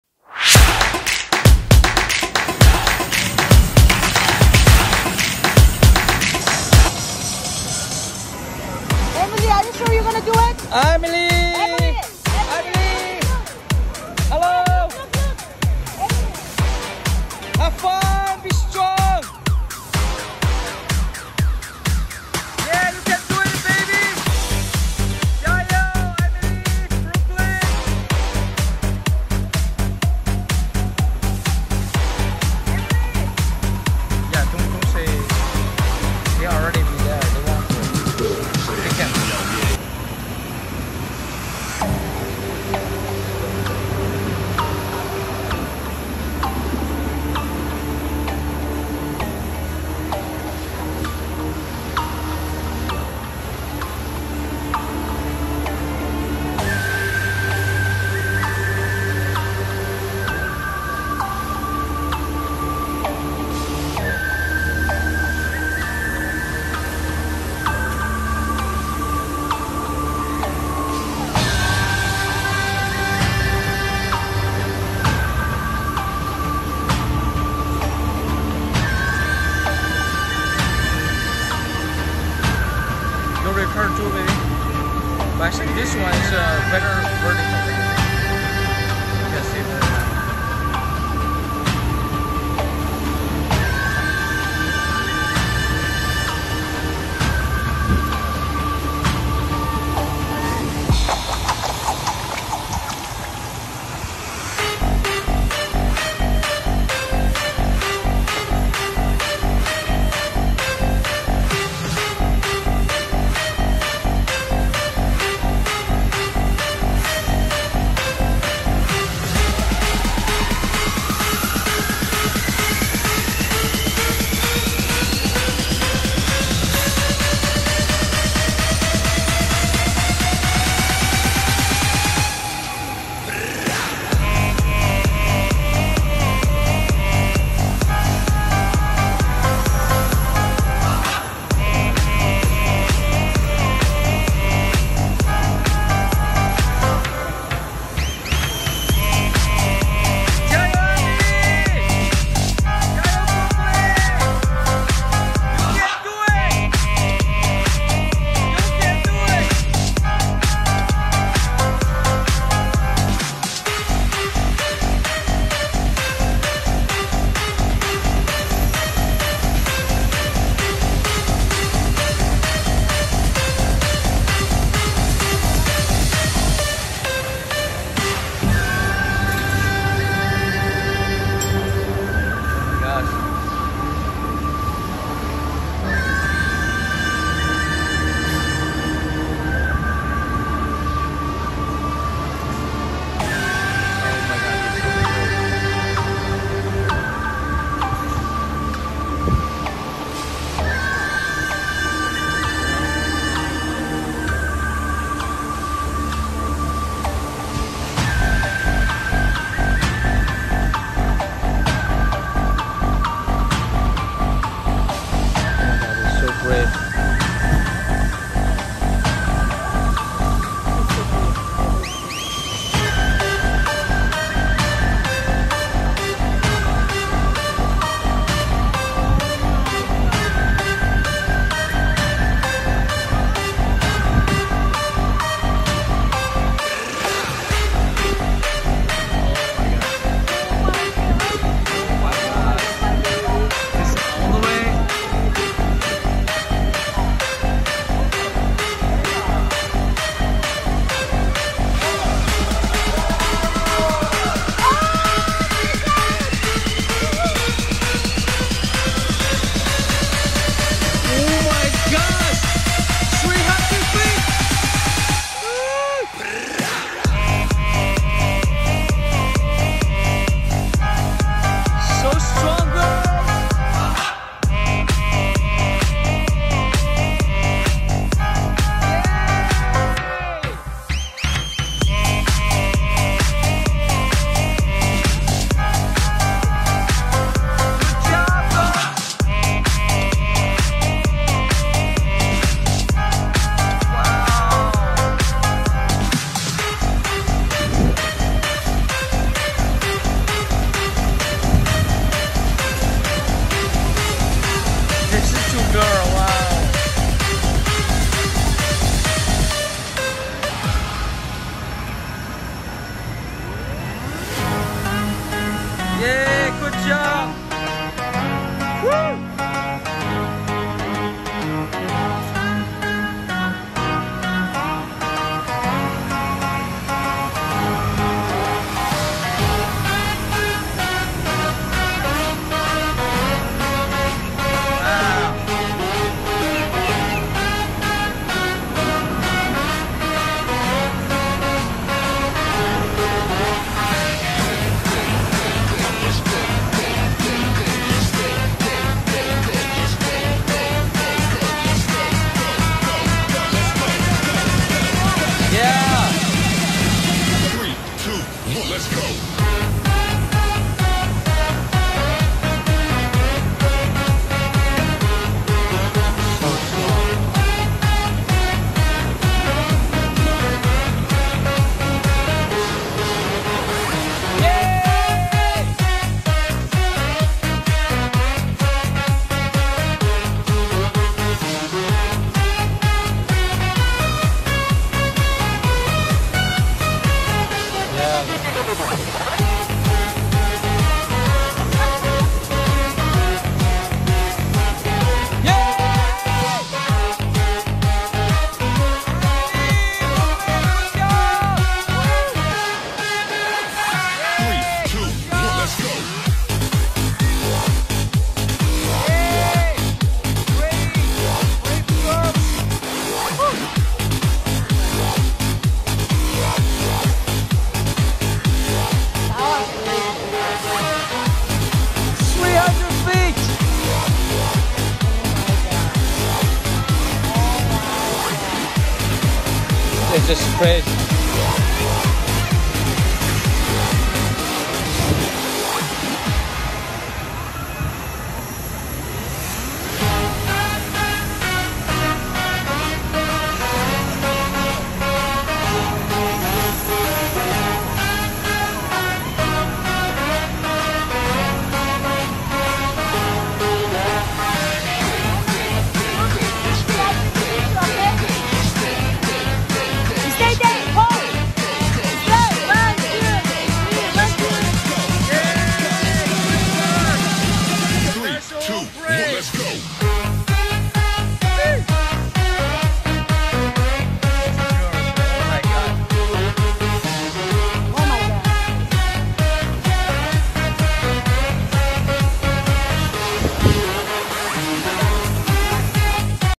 World’s tallest sky coaster / old town , Orlando , FL / emily & Brooklyn brave girls
For the ultimate thrill seeker!  Ride in groups of up to 3 people as you take flight on the World’s Tallest SkyCoaster standing at 300 feet! Get a great view of the attractions area and experience the closest thing to jumping out of a plane besides jumping out of a plane.